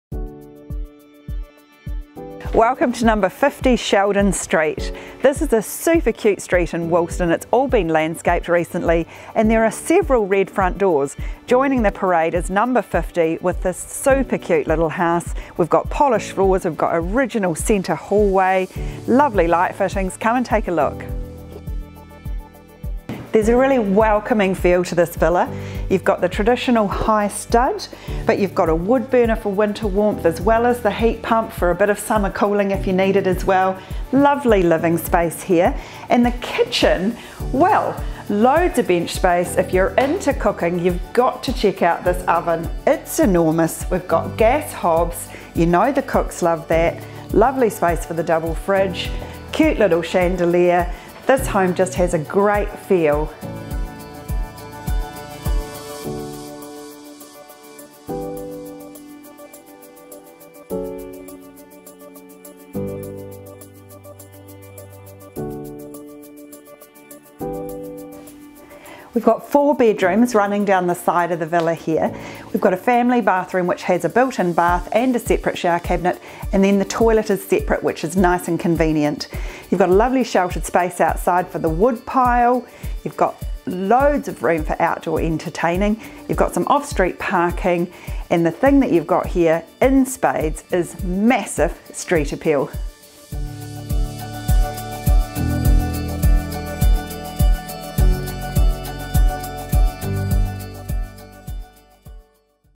Woolston 4 Bedroom Cutie
Gorgeous first home villa in lovely condition with open plan living, rewired, reroofed, woodburner, heat pump, insulated, freehold section.  Offers over $495k  First open home Sunday March 7th